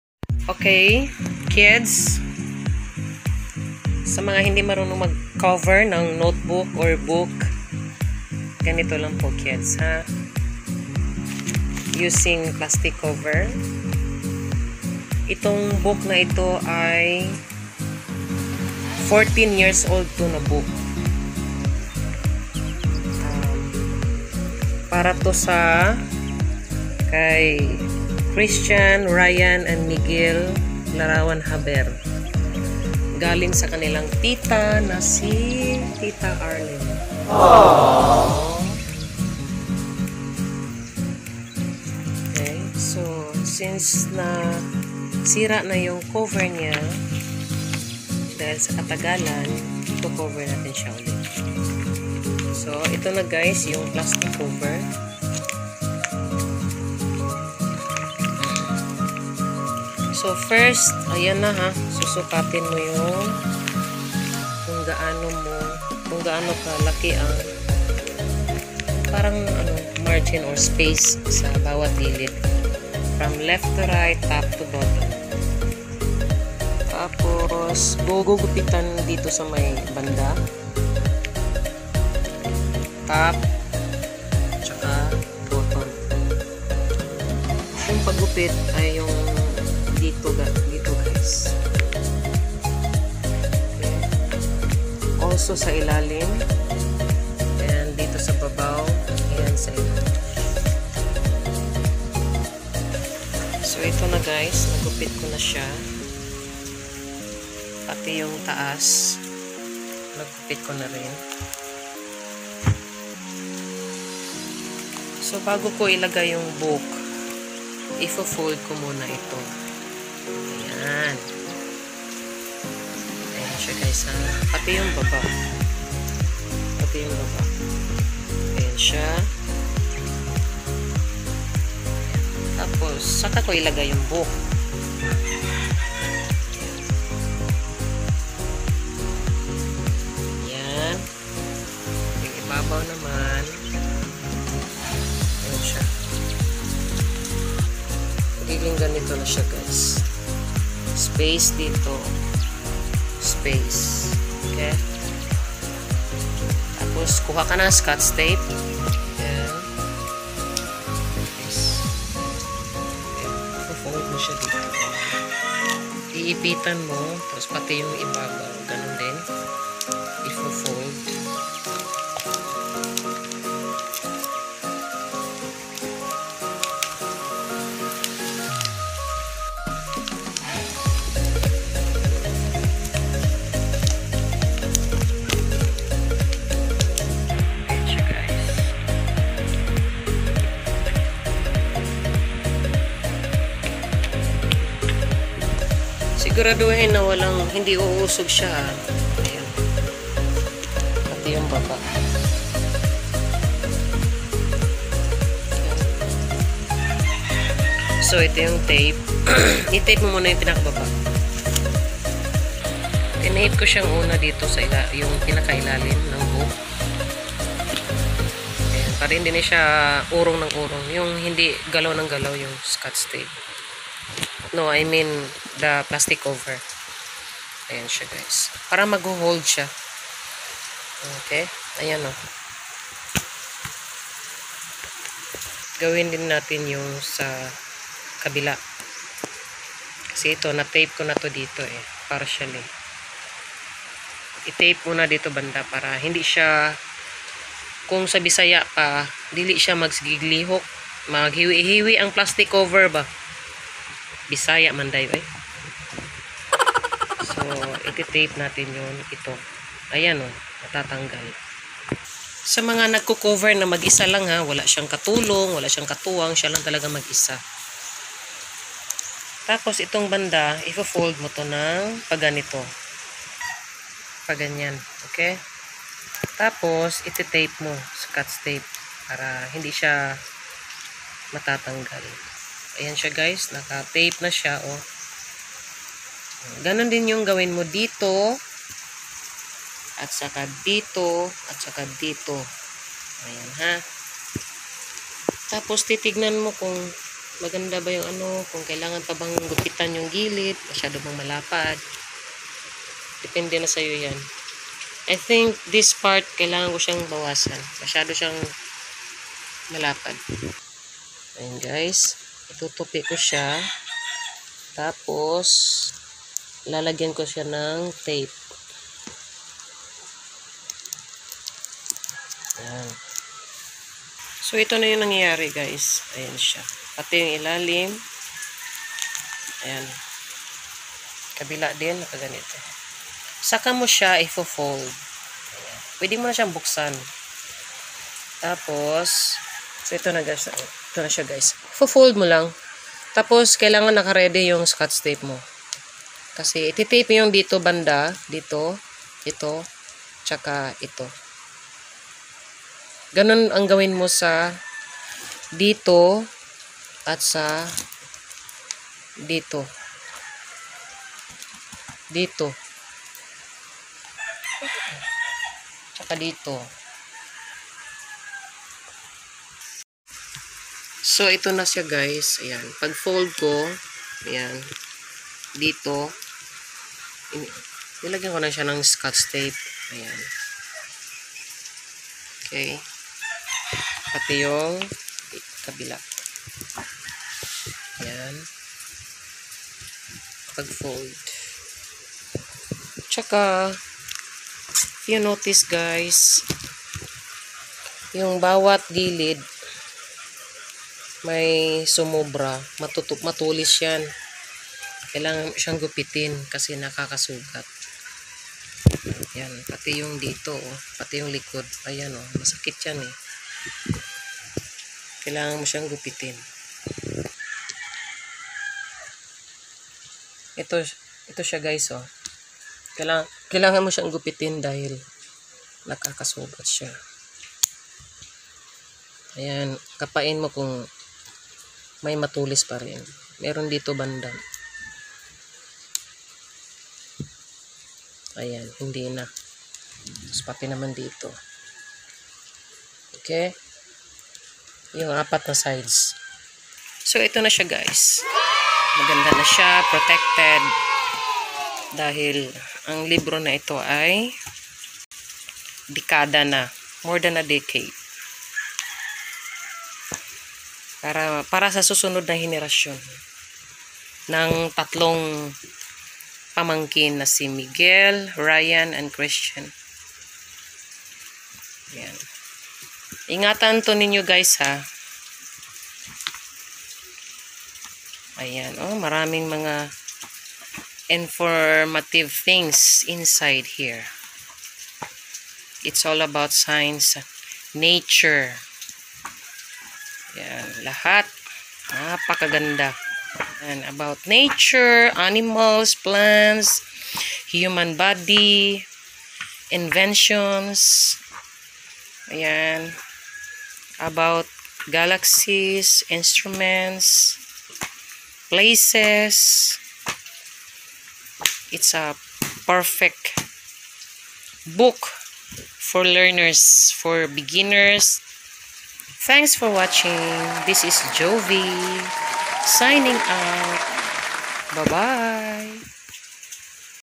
HOW TO COVER A BOOK, ETC.
Sa mga apo kong hirap gawin ang pagka cover ng mga books ninyo at pati na ng notebooks ninyo. Ito oh gayahin niyo lang ito. Napakadali lang 'wag tamad tamad okay 🤨🤨🤨 😘😘😘 love you all - Lola Juvy L. Haber ni 🥰